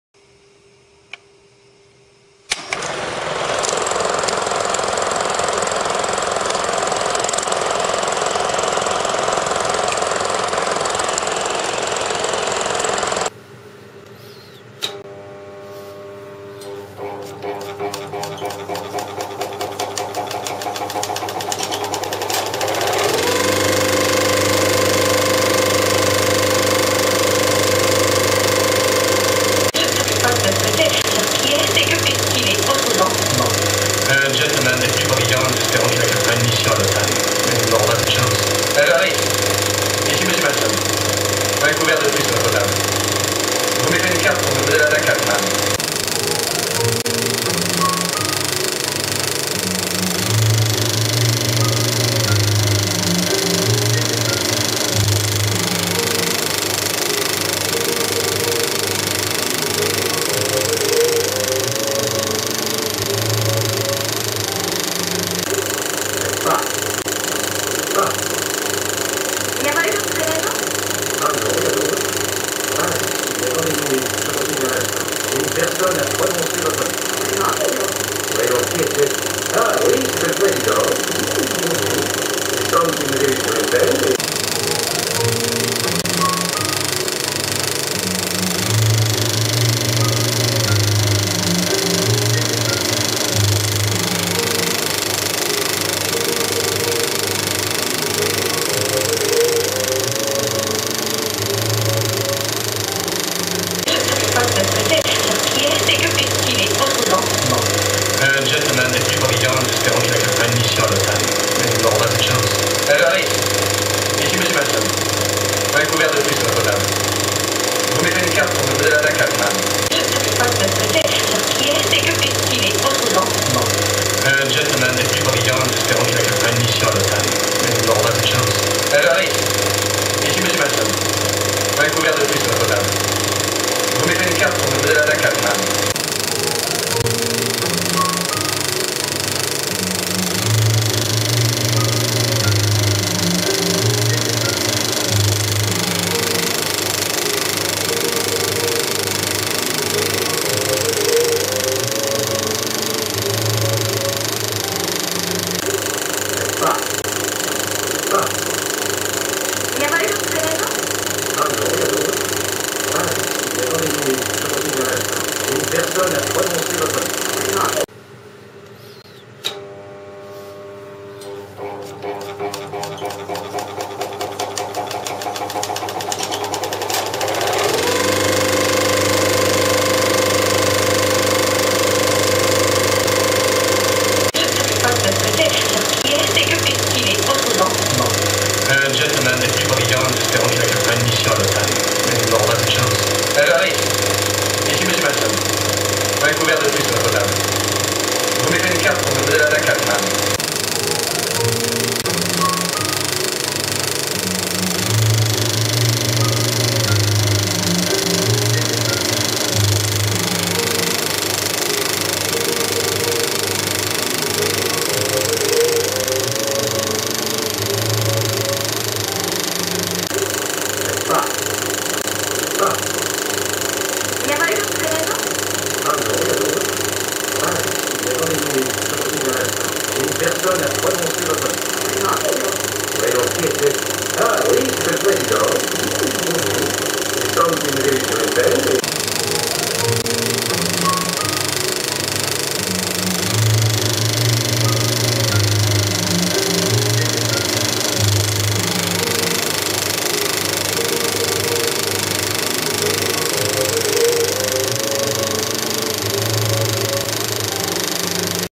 bruitage gratuit projection de film cinéma
Pour vos montages vidéos voici un enregistrement gratuit de bruitage réel de projecteur de cinéma 16mm et 35mm,ambiance projection de films en pellicule...comme dans une cabine de projectionniste.Merci de partager si vous aimez.